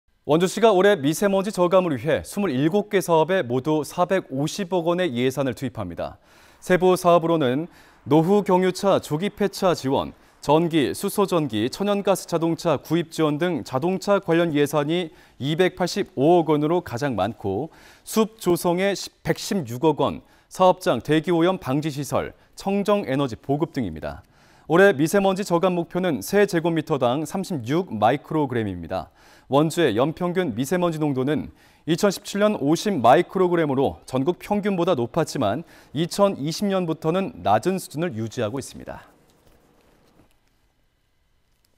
2023. 6. 5 [원주MBC] 원주시, 미세먼지 저감에 27개 사업 450억 투입
원주시가 올해 미세먼지 저감을 위해,
27개 사업에 모두 450억의 예산을 투입합니다.

세부사업으로는 노후경유차 조기폐차 지원,
전기·수소전기·천연가스 자동차 구입 지원 등
자동차 관련 예산이 285억으로 가장 많고,
숲조성 116억, 사업장 대기오염 방지시설,
청정에너지 보급 등입니다.

올해 미세먼지 저감목표는 세제곱미터 당
36마이크로 그램입니다.

원주의 연평균 미세먼지 농도는
2017년 50마이크로그램으로 전국평균보다
높았지만, 2020년부터는 낮은 수준을
유지하고 있습니다.